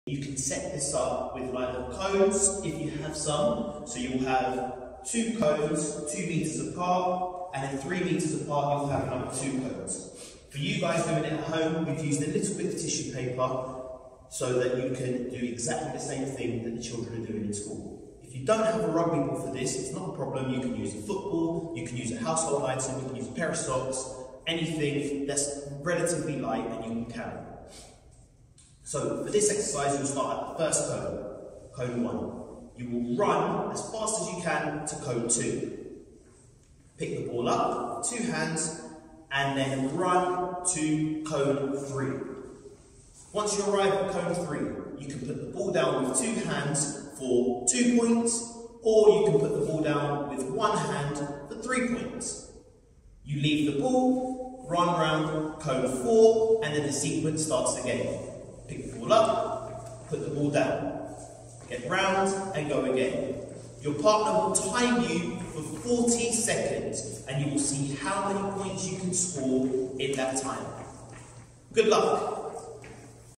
LYG Rugby League Challenges Stage 1 Activity 2 - Score a Try
Pupils have 40 seconds to score as many tries as they can.

Full competition rules and explanations are accessible below